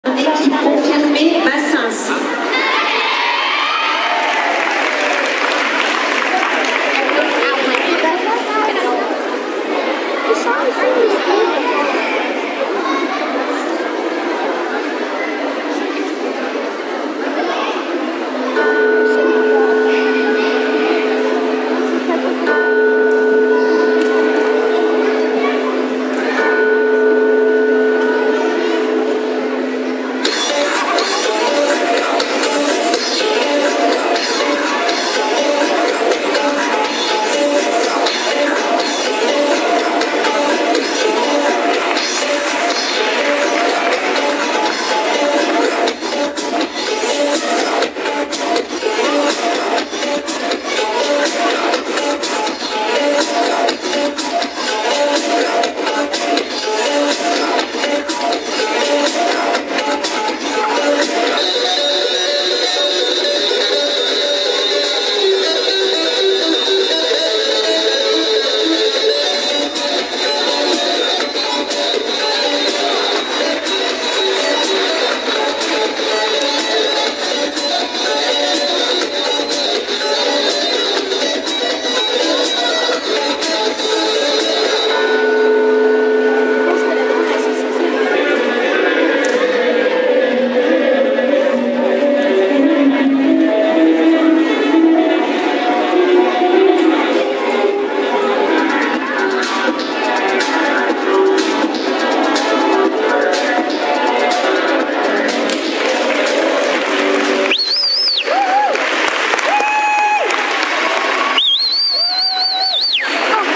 Chateauponsac GRS
Evenement de la gymnastique de Chateauponsac a La Souterraine